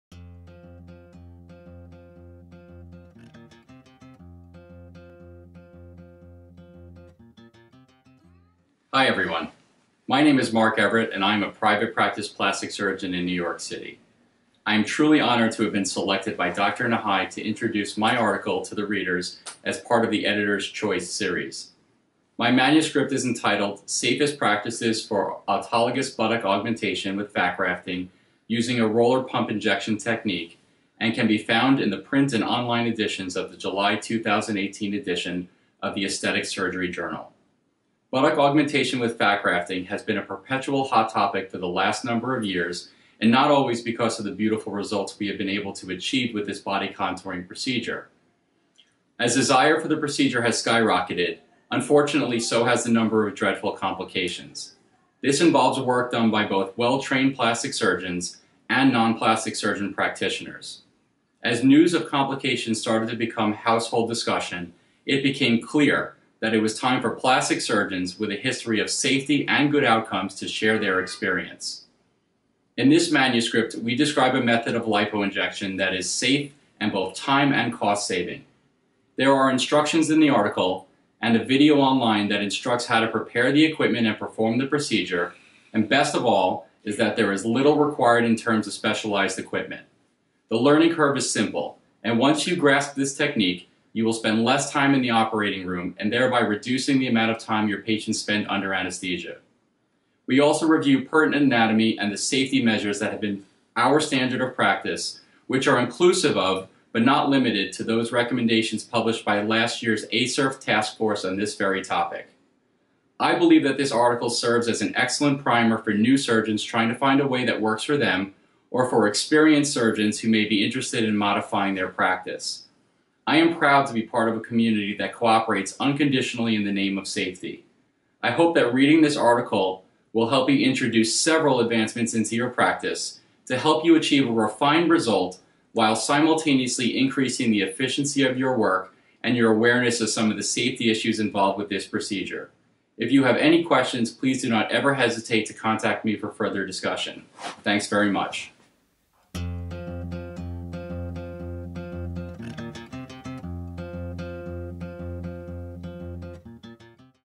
ASJ Article Highlight July 2018 with Dr. Marc Everett
In this article Dr. Marc Everett describes his article "Safest Practices for Autologous Buttock Augmentation with Fat Grafting Using a Roller Pump Injection Technique" in the July 2018 issue of the Aesthetic Surgery Journal. We hope you enjoy this exciting video and we encourage you to share it via Twitter and Facebook.

Twitter/Instagram: @ASJrnl; Facebook/LinkedIn: Aesthetic Surgery Journal